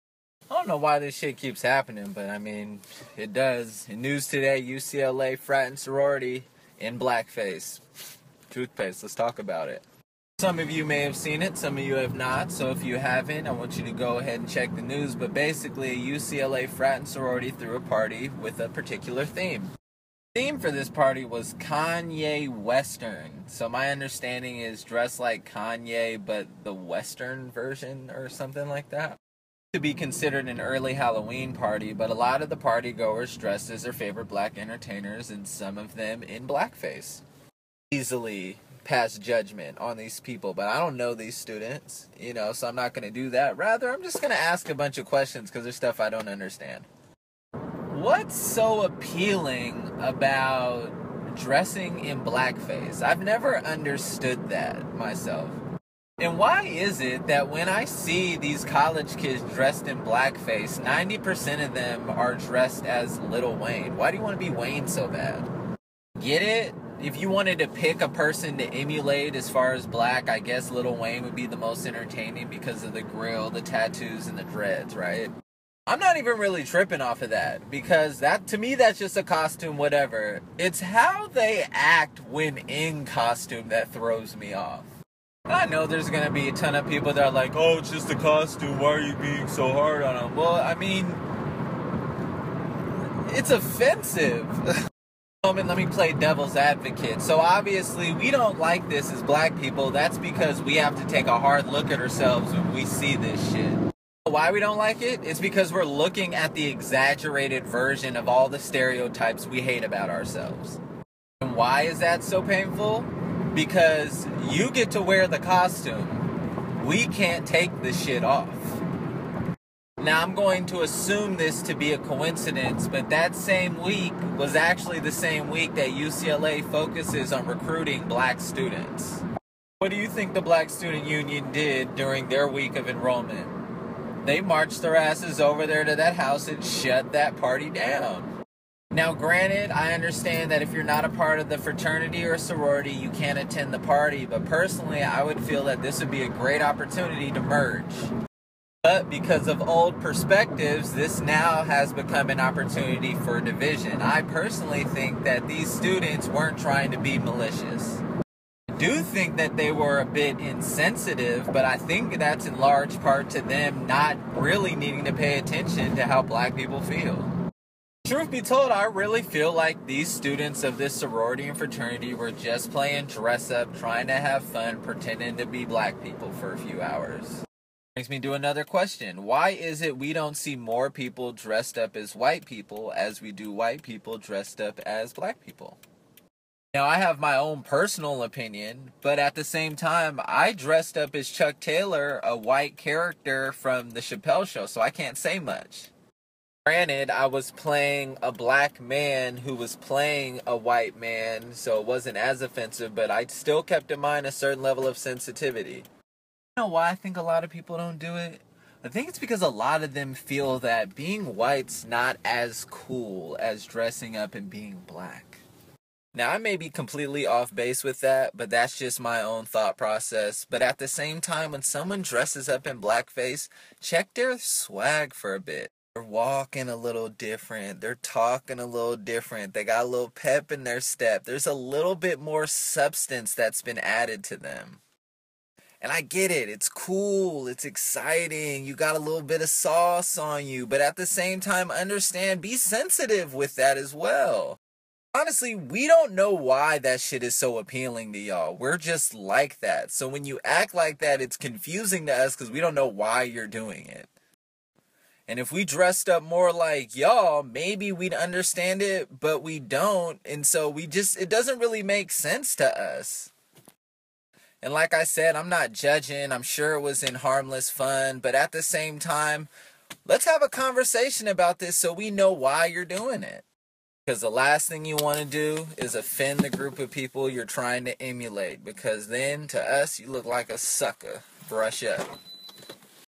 Truthpaste: UCLA Kanye Western Party
In this edition of Truthpaste I'm just asking a couple questions. Apparently a fraternity and Sorouty had a Kanye West-ern themed party and a couple people showed up in Black face. Didn't help this was during a week the university focuses on recruiting Black students either. Honest mistake or insensitive? Let's talk about it.